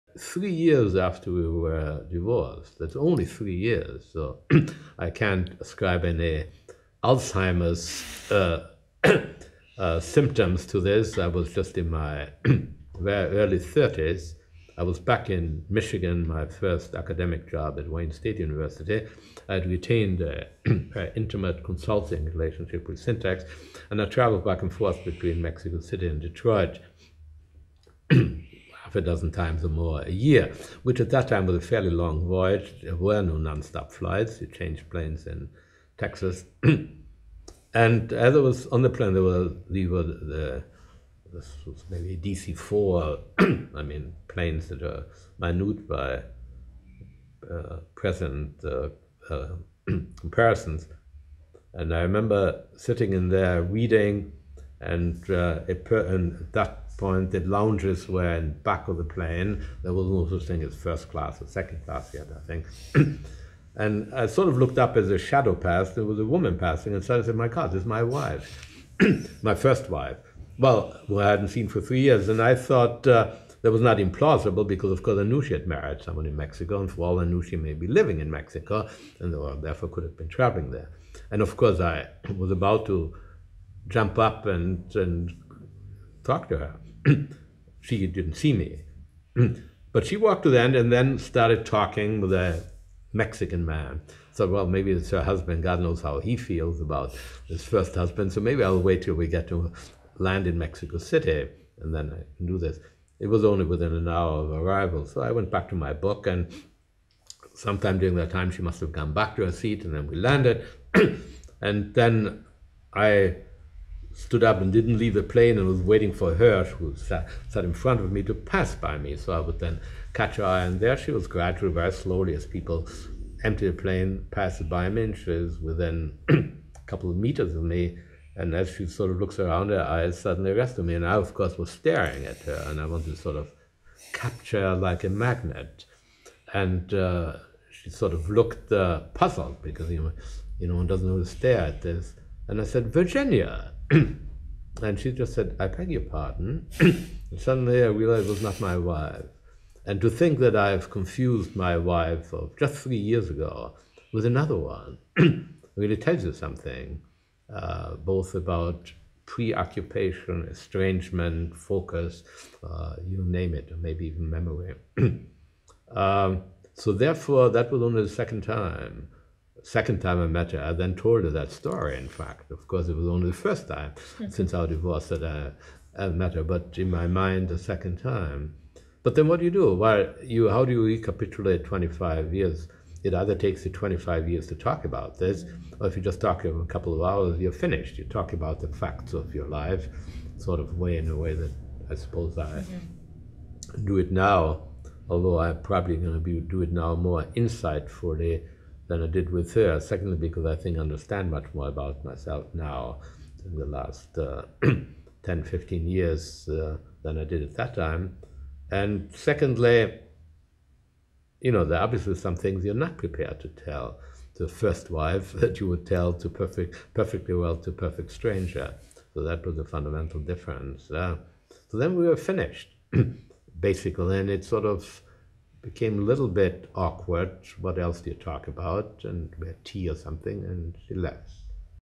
Carl Djerassi - Meeting my first wife after 25 years (10/117)
Austrian-American Carl Djerassi (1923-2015) was born in Vienna. He revolutionised the field of female contraception when he and his team of chemists produced synthetic progesterone which led to the development of the first oral contraceptive pill. [Listener: Tamara Tracz; date recorded: 2005]

TRANSCRIPT: Three years after we were divorced... that’s only three years, so I can’t ascribe any Alzheimer’s symptoms to this, I was just in my very early thirties. I was back in Michigan, my first academic job at Wayne State University. I’d retained an intimate consulting relationship with Syntex and I travelled back and forth between Mexico City and Detroit half a dozen times or more a year, which at that time was a fairly long voyage. There were no non-stop flights. You changed planes in Texas. And as I was on the plane... maybe a DC4, I mean, planes that are minute by present comparisons, and I remember sitting in there reading and at that point the lounges were in the back in the plane. There was no such thing as first class or second class then I think, and I sort of looked up as a shadow passed, and it was a woman passing and so I said,'My God, it’s my wife...' my first wife who I hadn’t seen for three years. And I thought it was not implausible because of course I knew she had married someone in Mexico and for all I knew, she may be living in Mexico and therefore could have been travelling there. And, of course, I was about to jump up and talk to her. She didn’t see me, but she walked to the end and then started talking with a Mexican man. I thought, well, maybe it’s her husband, God knows how he feels about this first husband, so maybe I will wait until we get to land in Mexico City. And then I can do this. It was only within an hour of arrival.

So I went back to my book and sometime during that time she must have come back to her seat and then we landed, and then I stood up and didn’t leave the plane. I was waiting for her... she was sat in front of me, to pass by me so I would then catch her eye. And there she was, going very slowly as people emptied the plane, passed by me and she was within a couple of metres of me, and as she sort of looks around, her eyes suddenly rested on me, and I of course was staring at her and I wanted to sort of capture her like a magnet. And she, sort of, looked puzzled because, you know, one doesn’t normally stare. And I said... 'Virginia,' and she just said, 'I beg your pardon?' And suddenly I realised it was not my wife, and to think that I had confused my wife of just three years ago with another one, really tells you something, both about preoccupation, estrangement, focus... you name it, maybe even memory.

So, therefore, that was only the second time... the second time I met her and told her that story in fact. Of course, it was only the first time since our divorce that I had met her, but in my mind, a second time. But then what do you do? How do you recapitulate 25 years? It either takes you 25 years to talk about this, or if you’re just talking a couple of hours, you’re finished. You talk about the facts of your life, sort of in a way that I do it now, although I’ll probably do it now more insightfully than I did with her, certainly because I think I understand much more about myself now, the last 10 or 15 years, than I did at that time. And, secondly, you know there are obviously some things you’re not prepared to tell to the first wife that you would tell perfectly well to a perfect stranger, so that was a fundamental difference. So then we were finished basically and then it sort of became a little bit awkward. What else do you talk about? And we had tea or something, and she left.